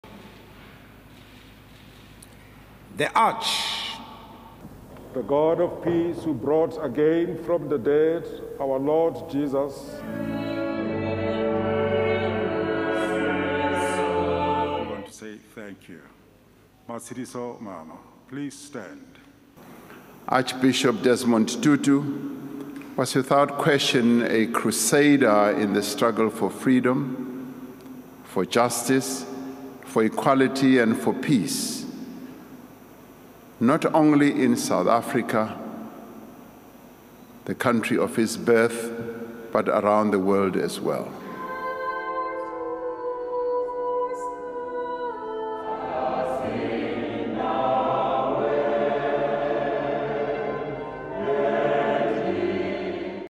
Desmond Tutu laid to rest in Cape Town
A funeral service has taken place for South Africa's Archbishop Desmond Tutu, who died last weekend at the age of 90. (Jan 1.)